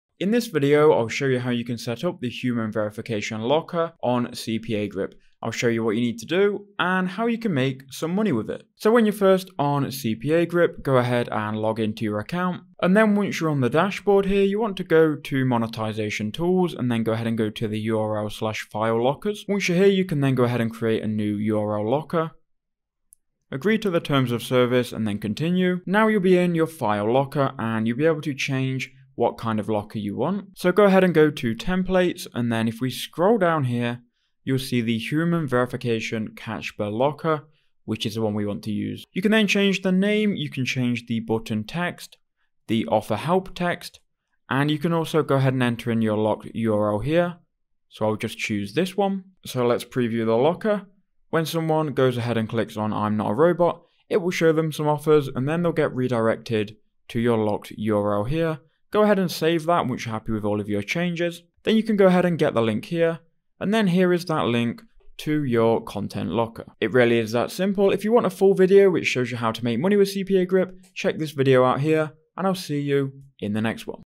CPAGrip Human Verification Locker Tutorial | 'I'm not a robot' Content Locker
CPAGrip Human Verification Locker Tutorial | 'I'm Not a Robot' Content Locker. This 'I'm not a robot' captcha is converting amazing! Learn how to get the CPA locker setup after watching this video!